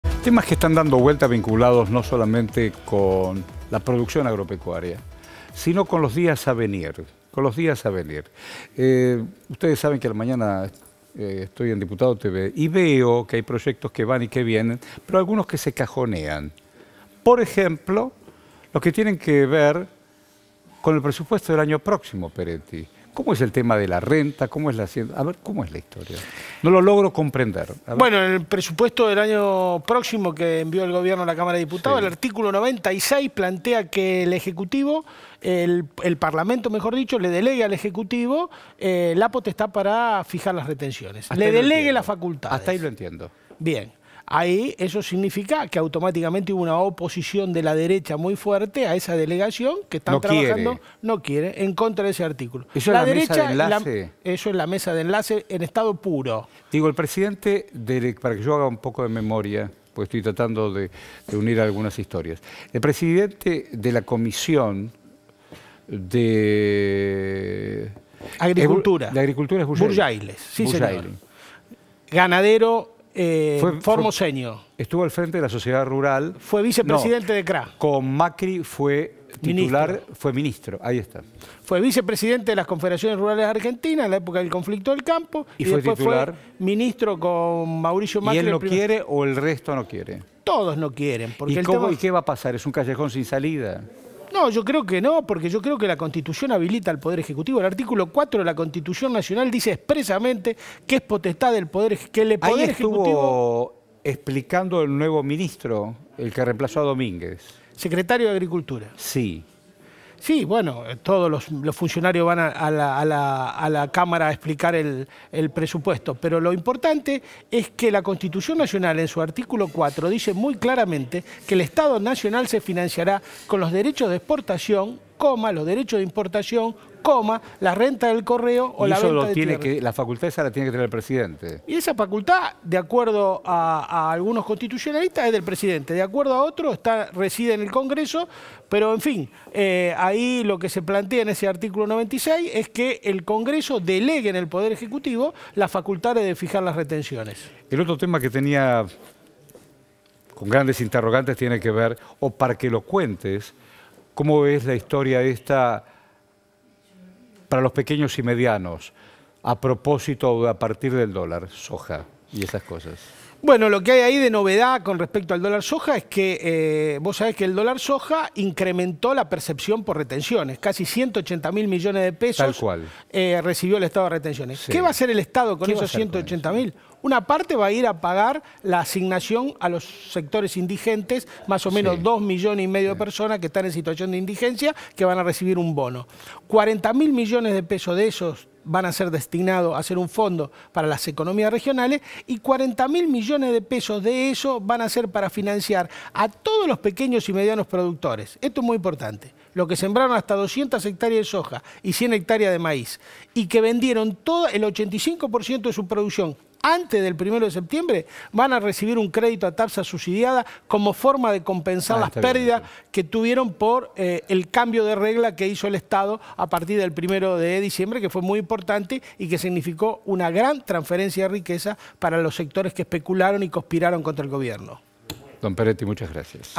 Presupuesto 2023 y ayuda a productores
El Gobierno podría establecer derechos de exportación, algo que rechaza la Federación Agraria. La ayuda a pequeños y medianos productores a través del "programa de fortalecimiento". La columna de Pedro Peretti, ex dirigentes de la FAA.